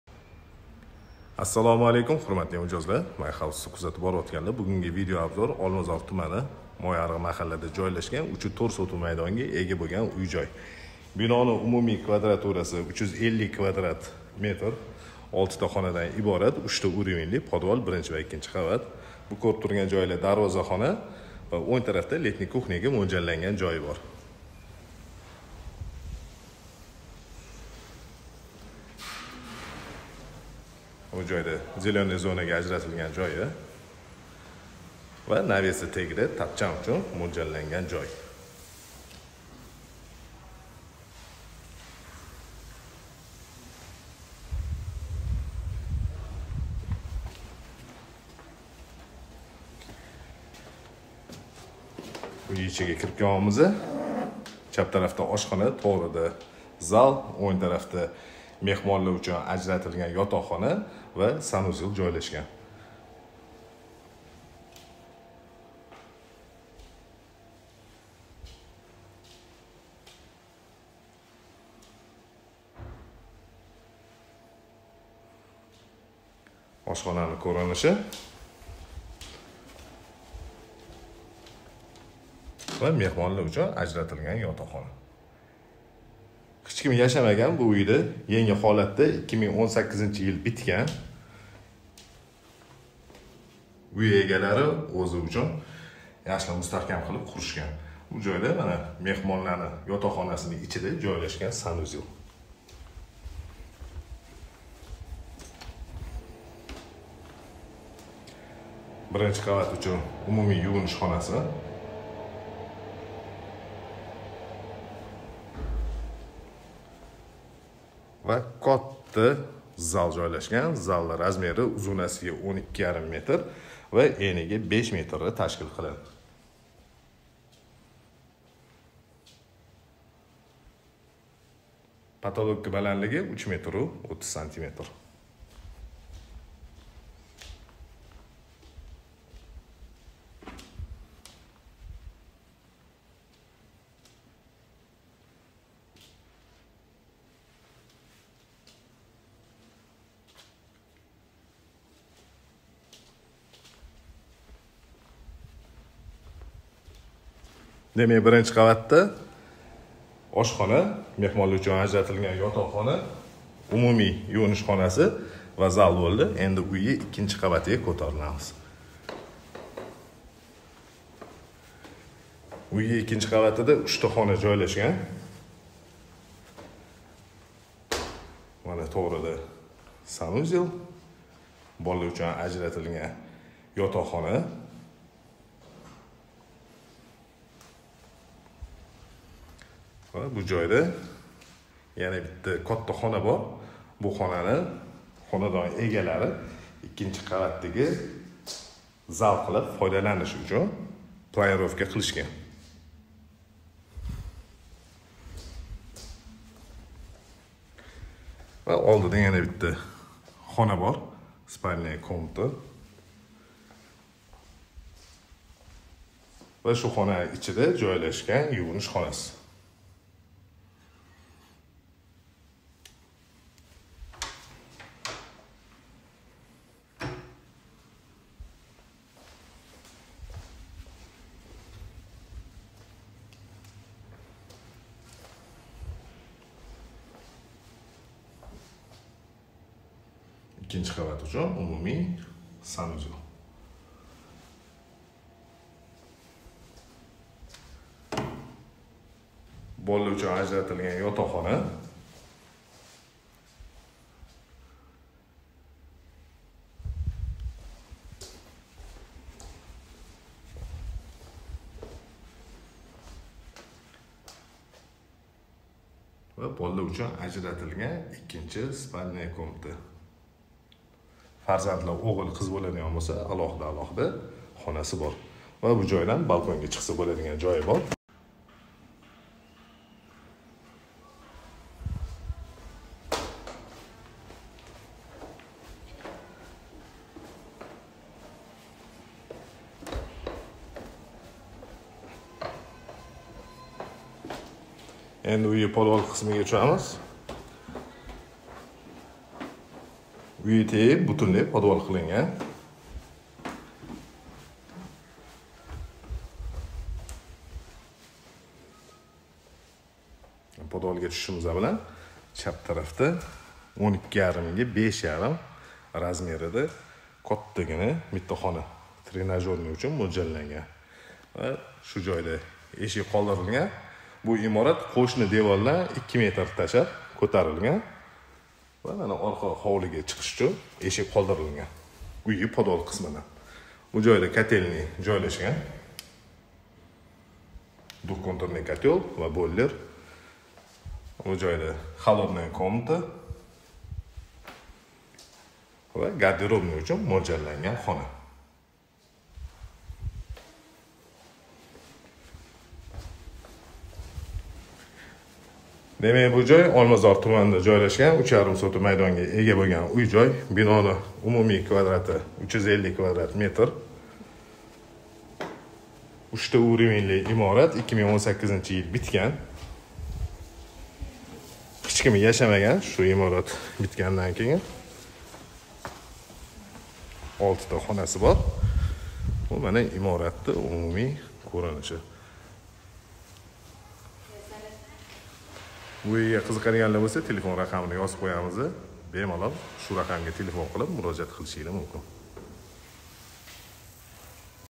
Новый Евро Дом 3,4 соток 6 комнат Алмазар Мойарык 320000$
Продаётся    новый евро дом !

Адрес: Алмазарский  район

Ориентир: Мойарик

Параметры:

Участок:3,4 соток

Комнат:6

Санузел: 4

Подвал под всем домом

Общая квадратура:350 кв.м

Цена:320.000$ торг уместен

Агарда уй сизга ёқан бўлса яқинларизга юборинг.✅
реклама учун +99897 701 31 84